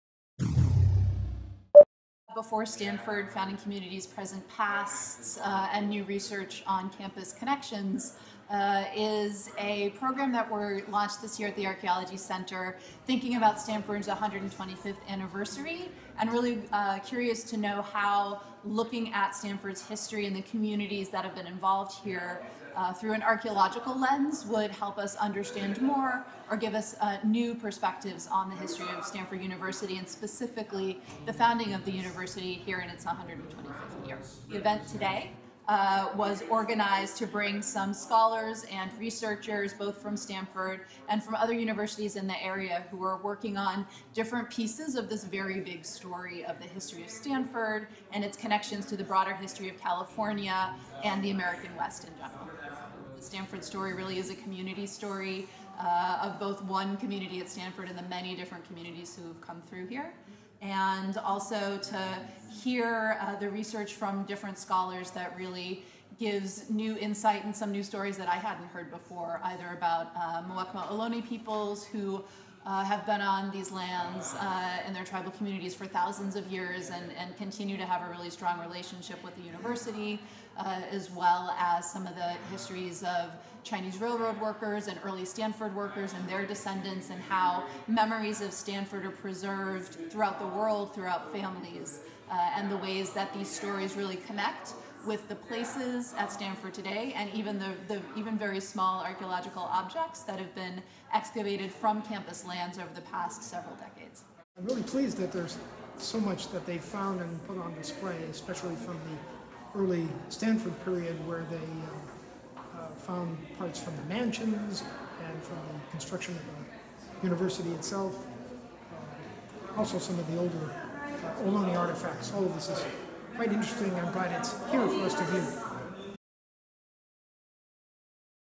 Before Stanford: Founding Communities, Present Pasts
Researchers reveal Stanford's hidden history at an open house for Stanford Archeology Center's new exhibit, "Before Stanford: Founding Communities, Present Pasts," on Friday, Oct. 21 (CAROLINE GAO/The Stanford Daily).